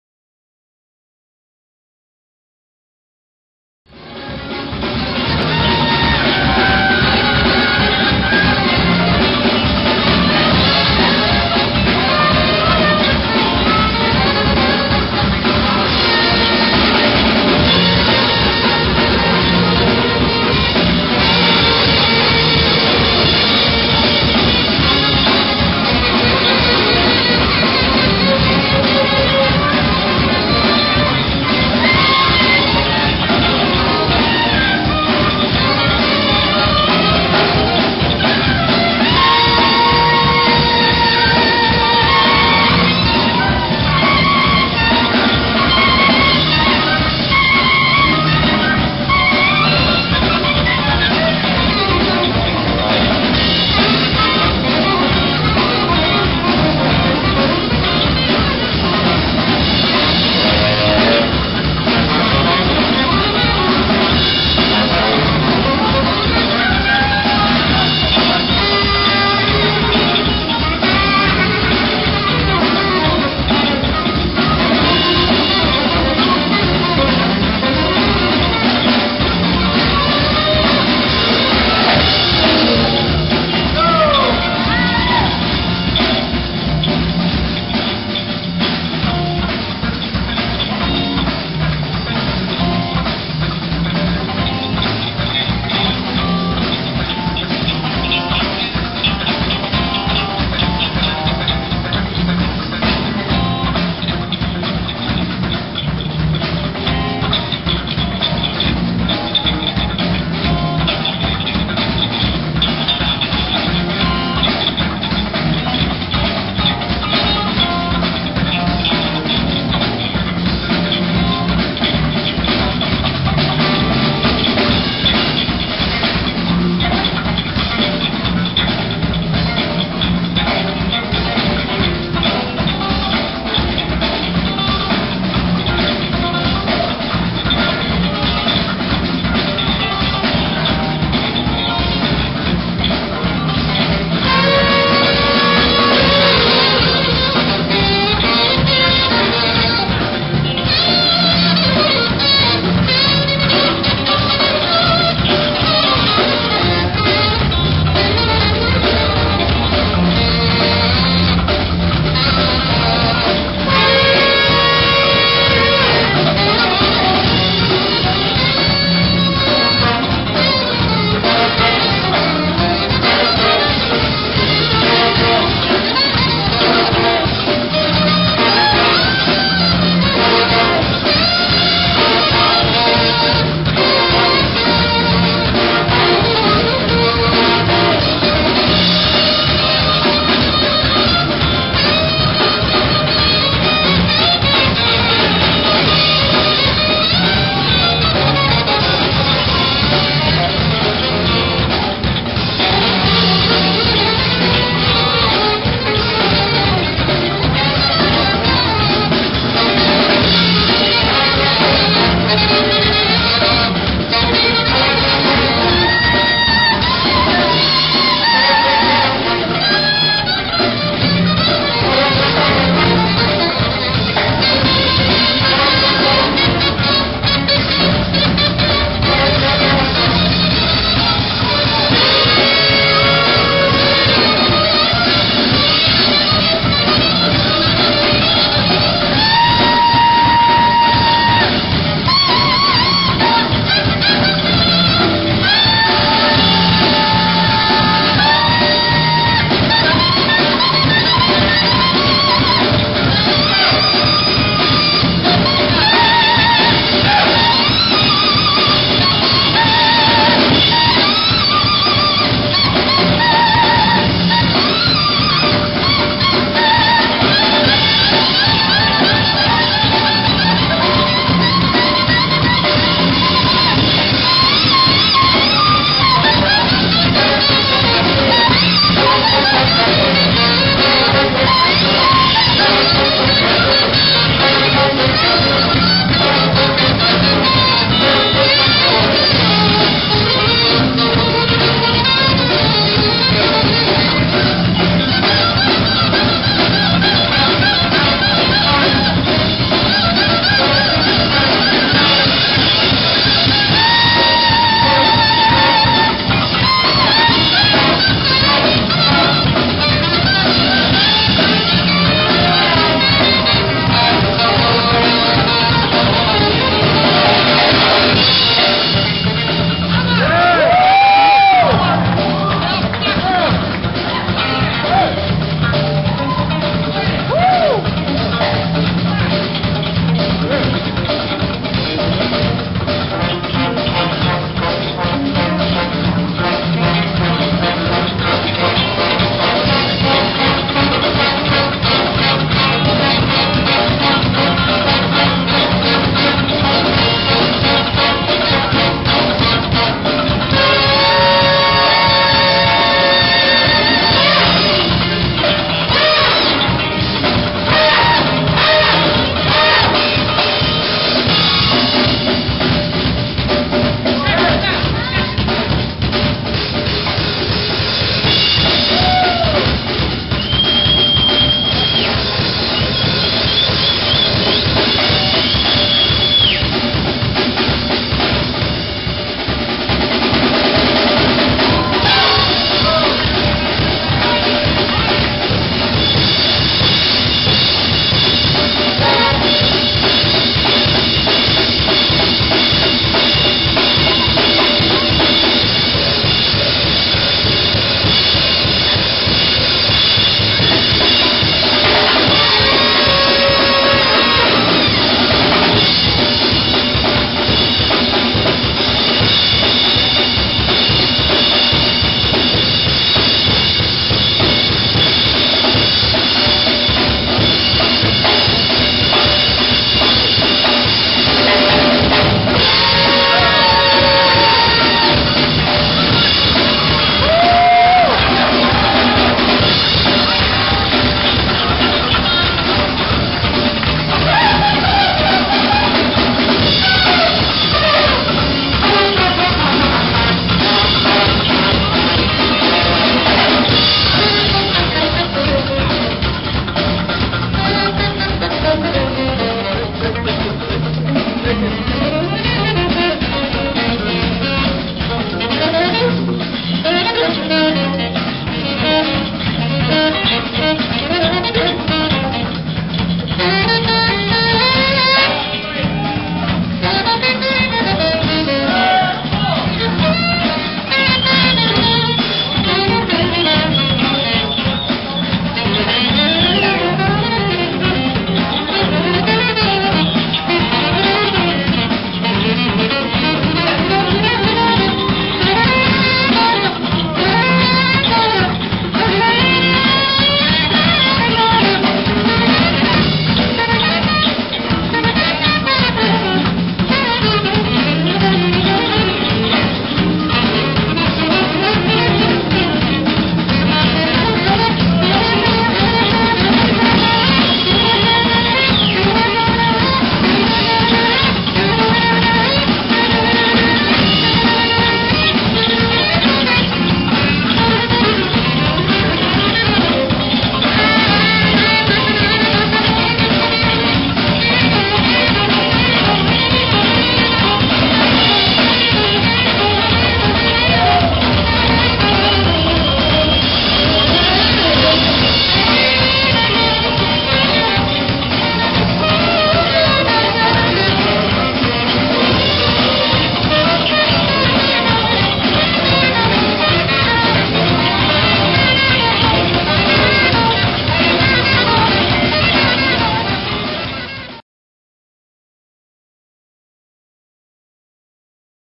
This is New York Alex Lo Dico Ensemble (caught on camera)
Got off the train and saw this band. They kick ass and you know what?--they play weddings--I thought that was weird...